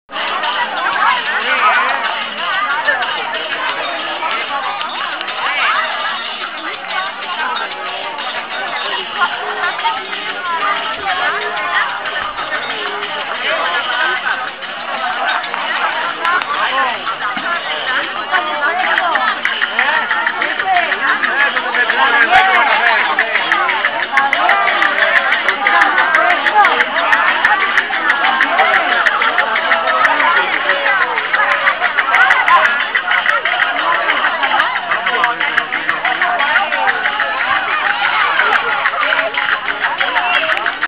Comares, Romería de San Isidro 2012. En el Balcón de la Axarquía (II).
Comares, Romería de San Isidro 2012. En el Balcón de la Axarquía. Después de la Santa Misa en honor a San Isidro labrador que tuvo lugar en la Iglesia de Nuestra Señora de la Encarnación.